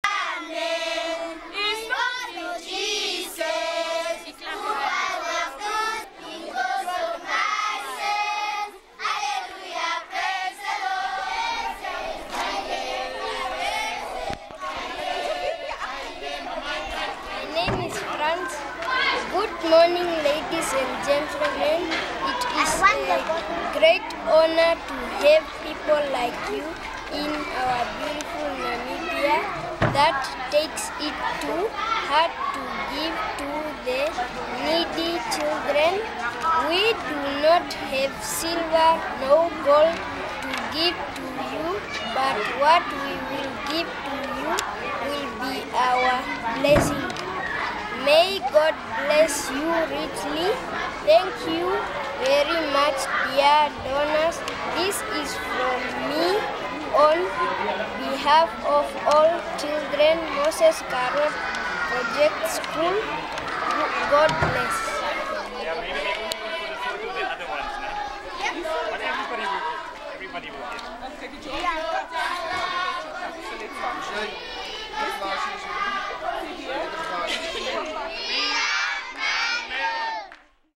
Namibia Dairies brings “local goodness” to Moses Garoeb project school in Havanna
Namibia Dairies – a subsidiary of the Ohlthaver & List (O&L) Group – recently brought smiles and even a few milk moustaches to the excited faces of approximately 1200 children of the Moses Garoëb Project School in the Havanna settlement of Katutura, Windhoek. Namibia Dairies not only provided the children with a glass of quality Nammilk Farm Fresh Longlife/UHT Milk, but also pledged its commitment to continue doing so twice a week, for the next four weeks.  The commitment was made in celebration of milk month which was celebrated globally during the month of June.